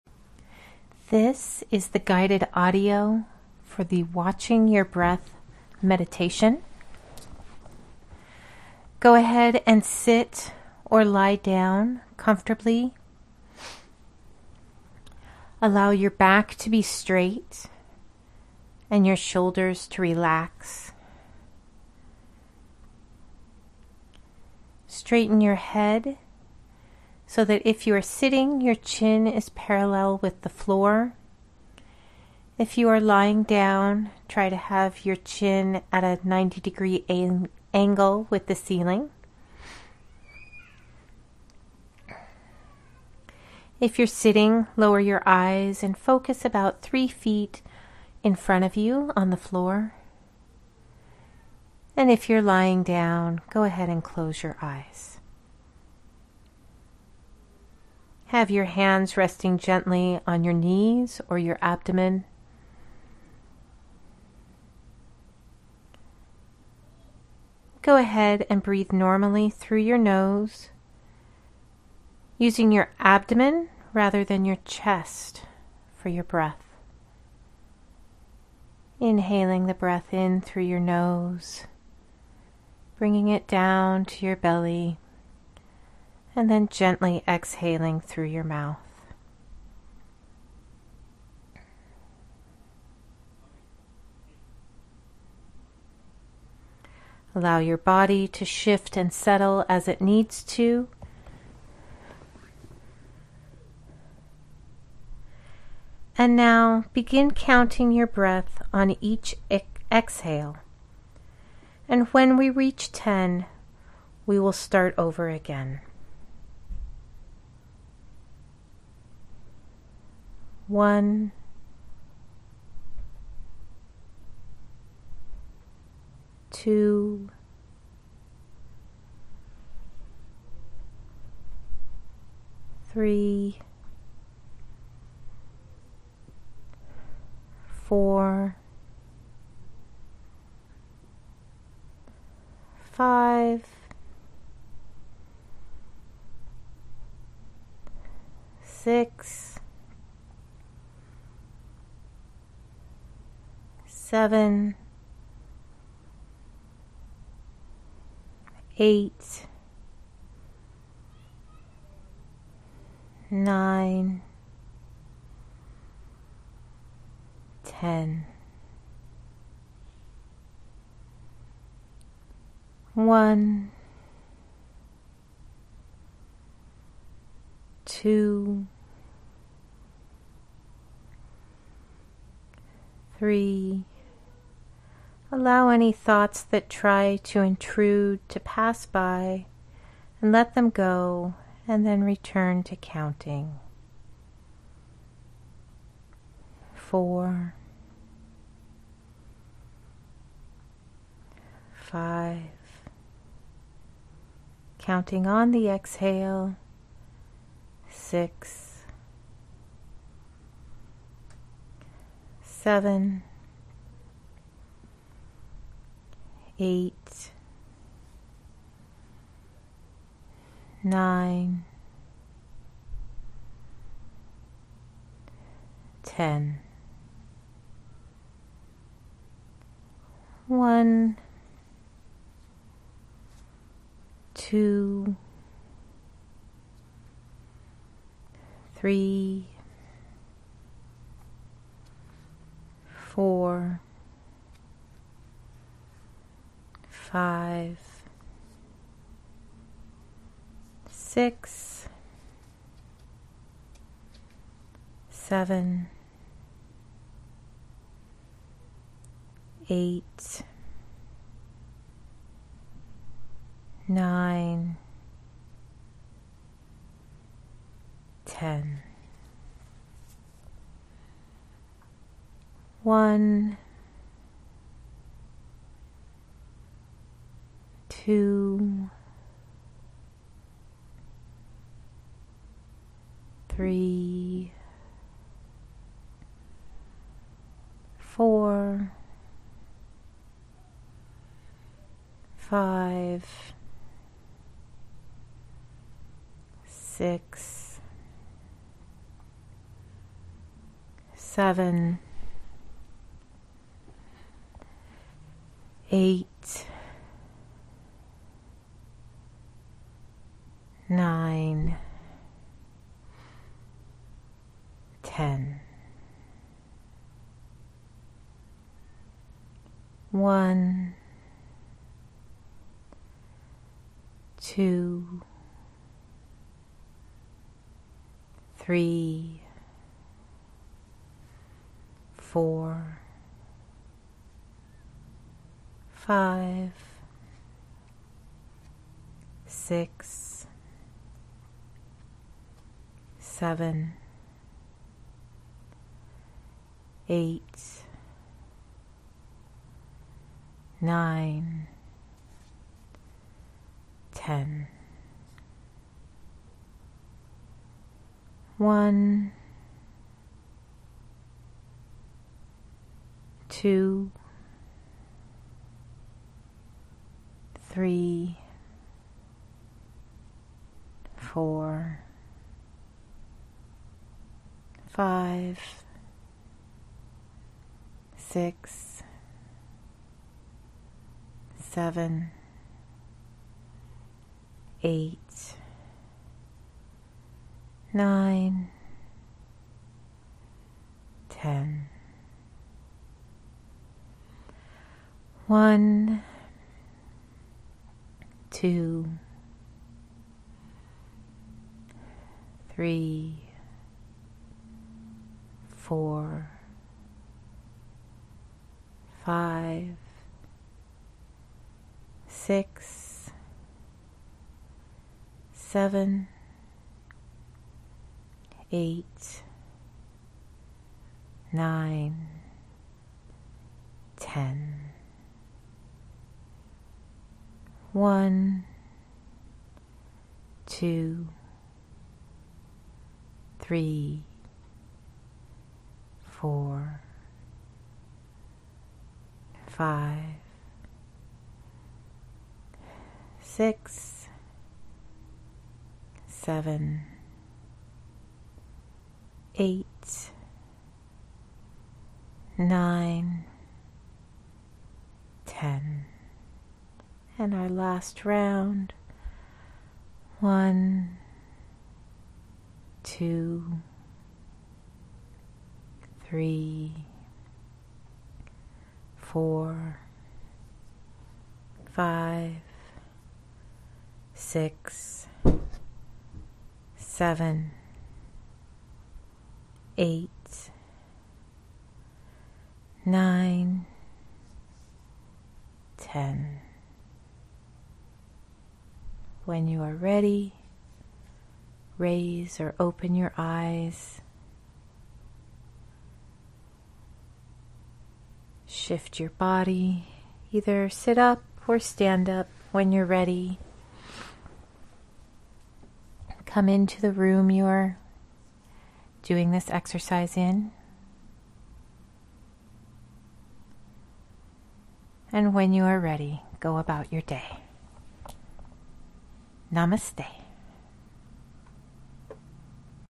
Watching your breath meditation
This meditation guides you in the practice of counting exhales in rounds of 10.